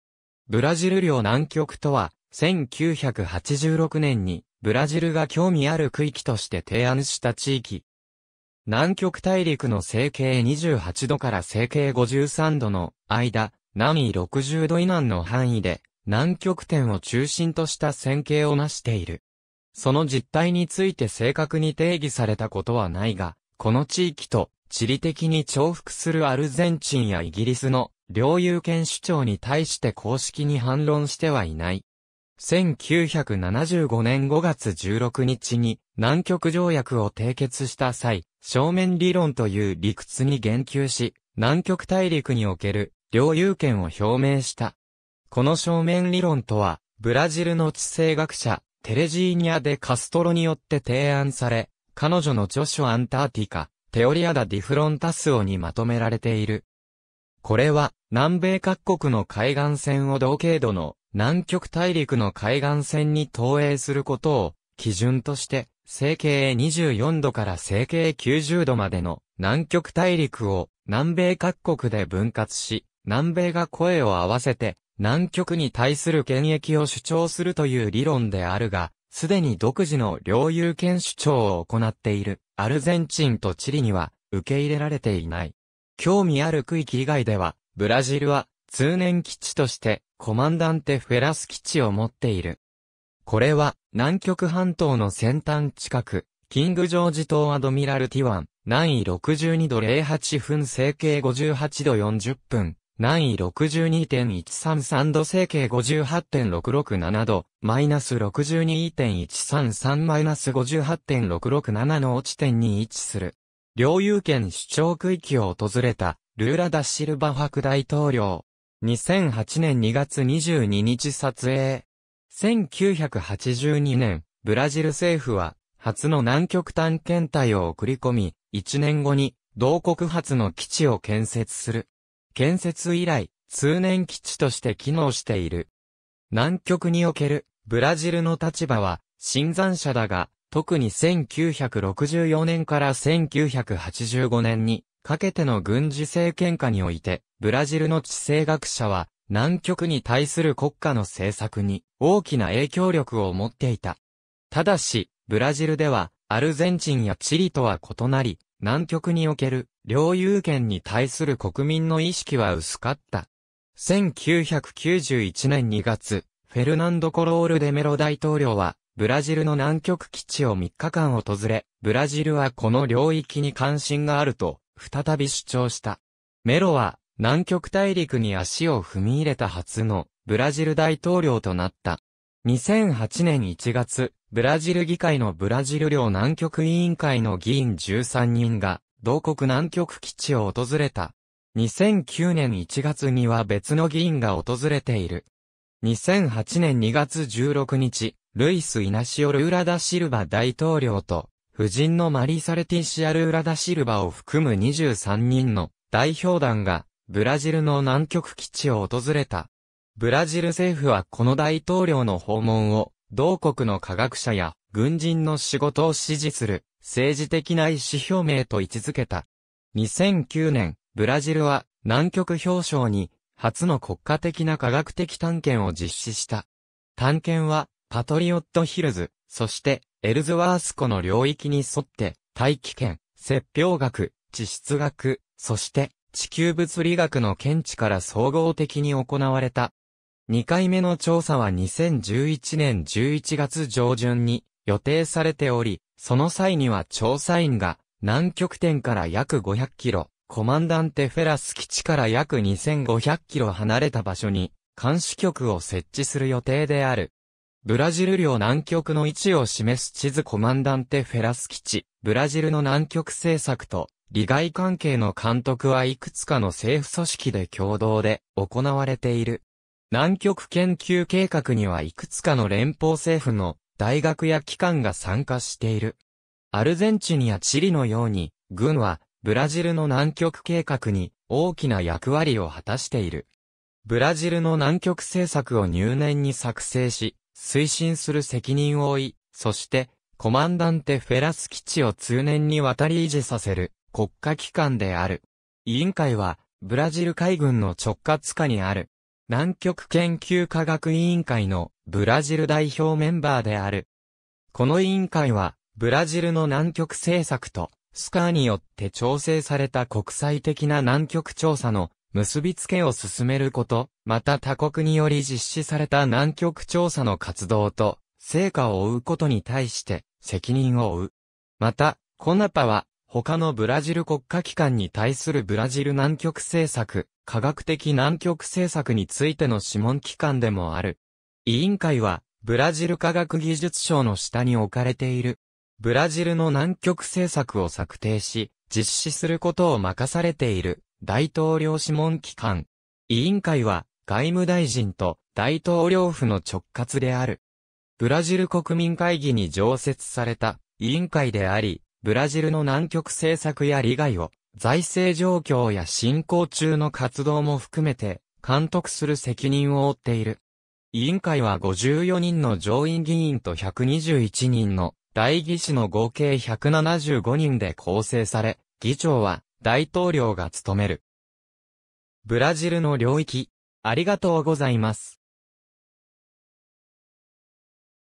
ブラジル領南極
ブラジル領南極（ブラジルりょうなんきょく、ポルトガル語: Antártida BrasileiraもしくはAntártica Brasileira）とは、1986年にブラジルが興味ある区域（「利益圏」）として提案した地域。
南極大陸の西経28度から西経53度の間、南緯60度以南の範囲(341,000km²)で南極点を中心とした扇形をなしている。
その実態について正確に定義されたことはないが、この地域と地理的に重複するアルゼンチンやイギリスの領有権主張に対して公式に反論してはいない。
1975年5月16日に南極条約を締結した際、「正面理論」という理屈に言及し、南極大陸における領有権を表明した。
この正面理論(Teoria da Defrontação)とは、ブラジルの地政学者テレジーニャ・デ・カストロによって提案され、彼女の著書Antártica: Teoria da Defrontaçãoにまとめられている。
これは南米各国の海岸線を同経度の南極大陸の海岸線に投影することを基準として、西経24度から西経90度までの南極大陸を南米各国で分割し、南米が声を合わせて南極に対する権益を主張するという理論であるが、すでに独自の領有権主張を行っているアルゼンチンとチリには受け入れられていない。
「興味ある区域」以外では、ブラジルは通年基地としてコマンダンテ・フェラス基地（UN/LOCODE：AQ-CFZ）を持っている。
これは南極半島の先端近く、キングジョージ島アドミラルティ湾、 南緯62度08分 西経58度40分 / 南緯62.133度 西経58.667度 / -62.133; -58.667の地点に位置する。
領有権主張区域を訪れたルーラ・ダ・シルヴァ伯大統領。
2008年2月22日撮影。
1982年、ブラジル政府は初の南極探検隊を送り込み、1年後に同国初の基地を建設する（南極大陸での活動において死んだ海軍士官にちなみ、コマンダンテ・フェラスと名付けられた）。
建設以来、通年基地として機能している。
南極におけるブラジルの立場は新参者だが、特に1964年から1985年にかけての軍事政権下において、ブラジルの地政学者は南極に対する国家の政策に大きな影響力を持っていた。
ただし、ブラジルではアルゼンチンやチリとは異なり、南極における領有権に対する国民の意識は薄かった。
1991年2月、フェルナンド・コロール・デ・メロ大統領はブラジルの南極基地を3日間訪れ、ブラジルはこの領域に関心があると再び主張した。
メロは南極大陸に足を踏み入れた初のブラジル大統領となった。
2008年1月、ブラジル議会のブラジル領南極委員会の議員13人が同国南極基地を訪れた。
2009年1月には別の議員が訪れている。
2008年2月16日、ルイス・イナシオ・ルーラ・ダ・シルヴァ大統領と、夫人のマリサ・レティシア・ルーラ・ダ・シルヴァを含む23人の代表団がブラジルの南極基地を訪れた。
ブラジル政府はこの大統領の訪問を、同国の科学者や軍人の仕事を支持する「政治的な意思表明」と位置づけた。
2009年、ブラジルは南極氷床に初の国家的な科学的探検を実施した。
探検はパトリオットヒルズ、そしてエルズワース湖（氷底湖）の領域に沿って、大気圏、雪氷学、地質学、そして地球物理学の見地から総合的に行われた。
2回目の調査は2011年11月上旬に予定されており、その際には調査員が南極点から約500キロ、コマンダンテ・フェラス基地 (EACF) から約2,500キロ離れた場所に監視局（さらに上級の基地）を設置する予定である。
ブラジル領南極の位置を示す地図 コマンダンテ・フェラス基地 (EACF) ブラジルの南極政策と利害関係の監督はいくつかの政府組織で共同で行われている。
南極研究計画にはいくつかの連邦政府の大学や機関が参加している。
アルゼンチンやチリのように、軍（特にブラジル空軍とブラジル海軍）はブラジルの南極計画に大きな役割を果たしている。
（ポルトガル語: Comissão Interministerial para os Recursos do Mar）ブラジルの南極政策（Programa Antártico Brasileiro (PROANTAR)）を入念に作成し、推進する責任を負い、そしてコマンダンテ・フェラス基地を通年にわたり維持させる国家機関である。
委員会はブラジル海軍の直轄下にある。
（ポルトガル語: Comitê Naciona...